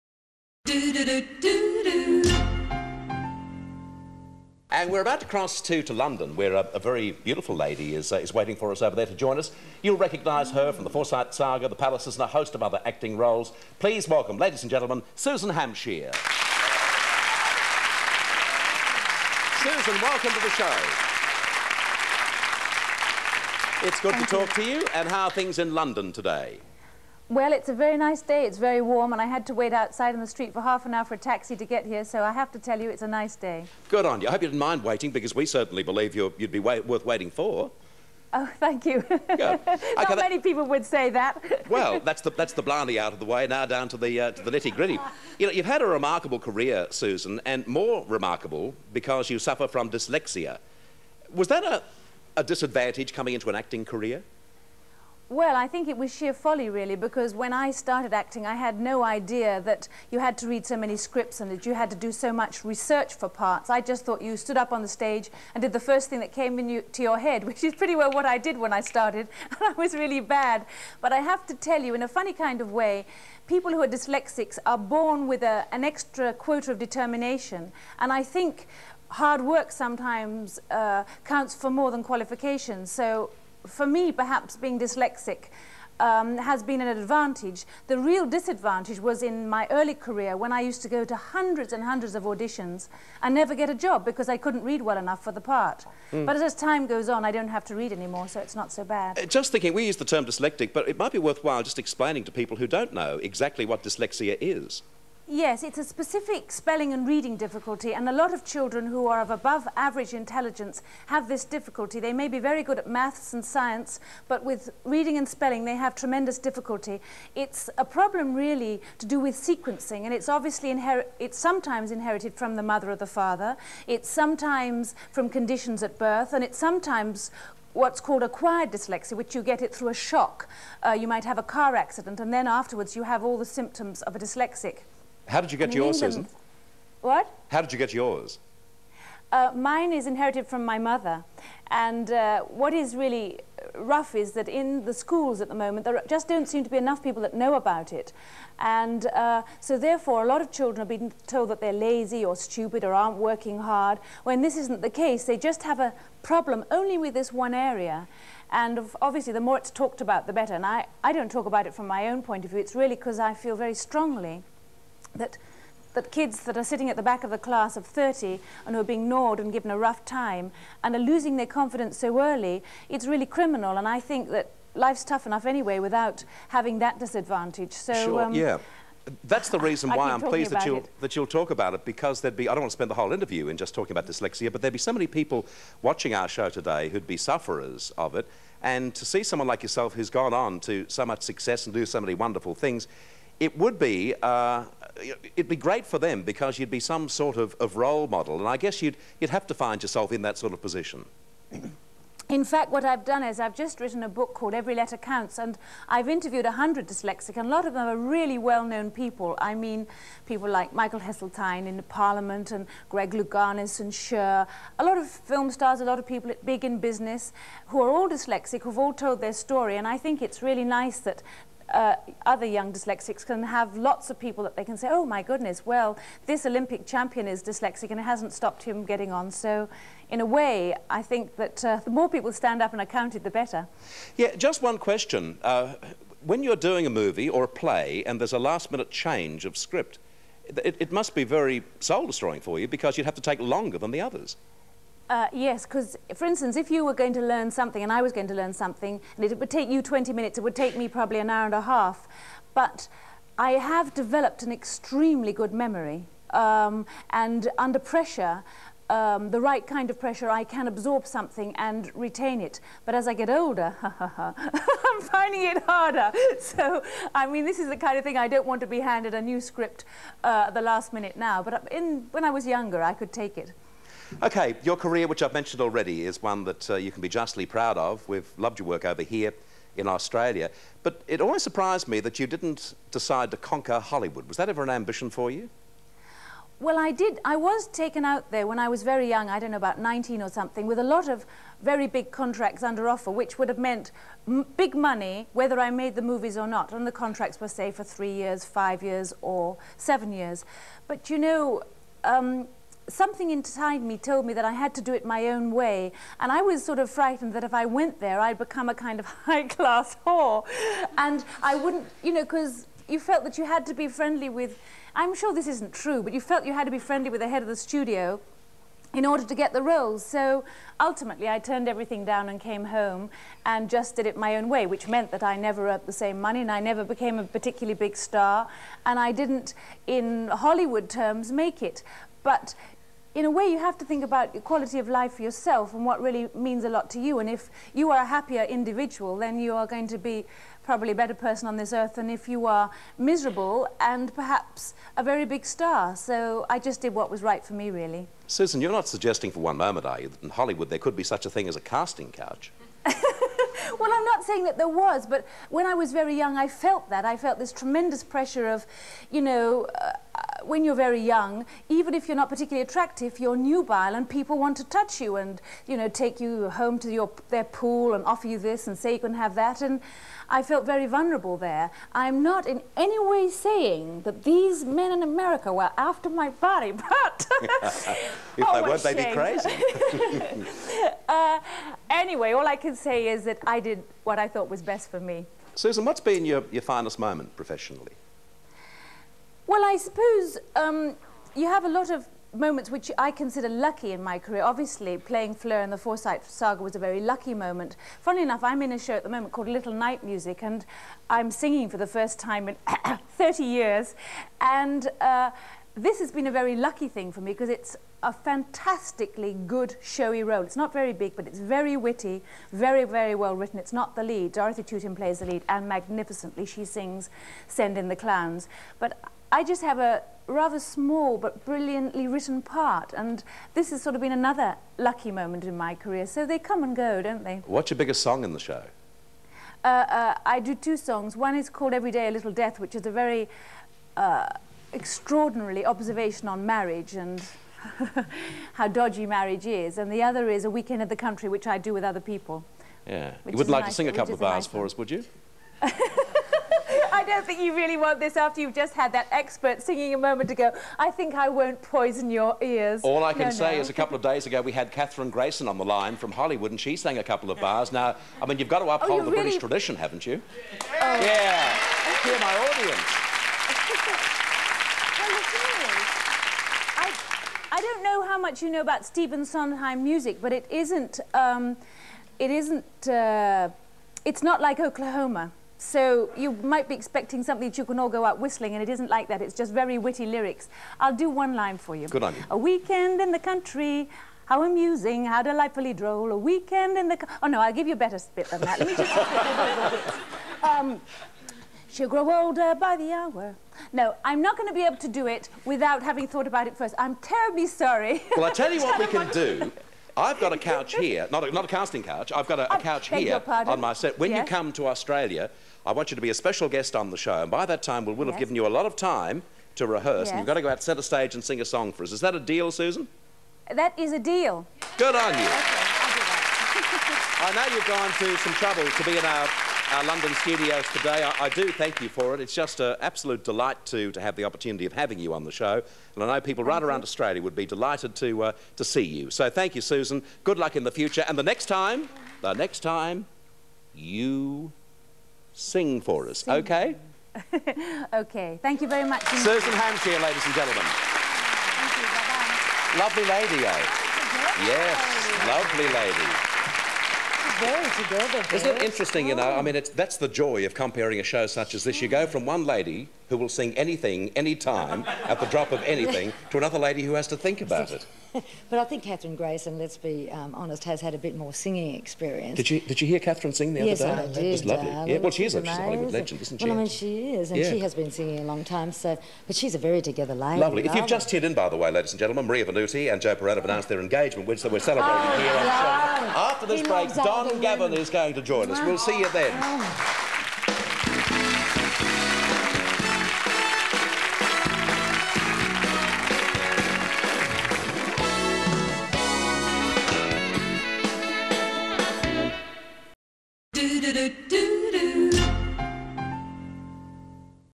Susan Hampshire tells Australian TV viewers about her dyslexia
Susan Hampshire, Lady Kulukundis, CBE is an English actress known for her many television and film roles. She is a three-time Emmy Award winner, winning for The Forsyte Saga in 1970, The First Churchills in 1969, and for Vanity Fair in 1973.
In this Australian television interview from 20 October, 1989, Susan speaks with Bert Newton about her career to that point and also opens up about her dyslexia.